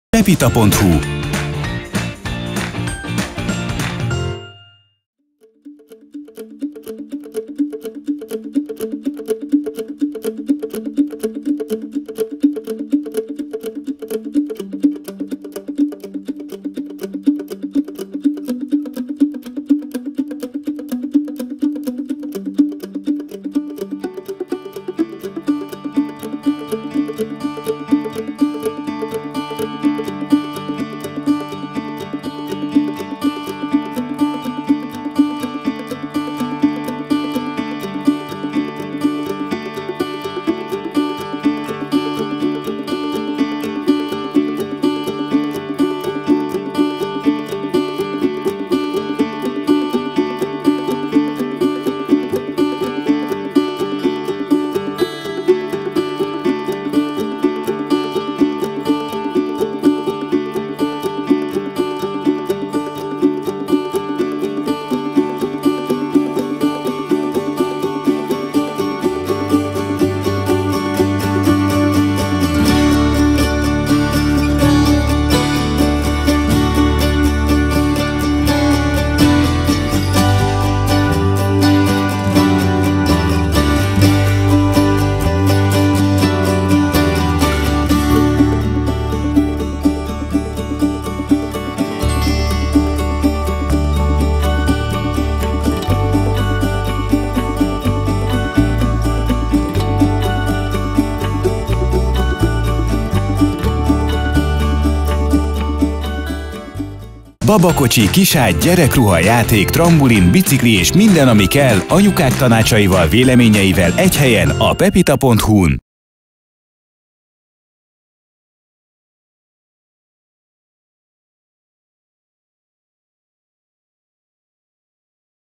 Espiro Magic sport babakocsi - Pepita.hu
Espiro Magic sport babakocsinál nagyobb kosár és lábtartó párosul a Magic-től megszokott modern kinézethez és egyszerű kezelhetőséghez! Ultrakönnyű vázszerkezetű, mely rendkívüli előny a sokat tömegközlekedők számára, ráadásul a lépcsőzések alkalmával sem lesz nehézkes a babakocsi cipelése. Extra kicsi méretűre, lapra összecsukható, akár repülőgépre is felvihető és a csomagtartóban, illetve otthon sem foglal sok helyet.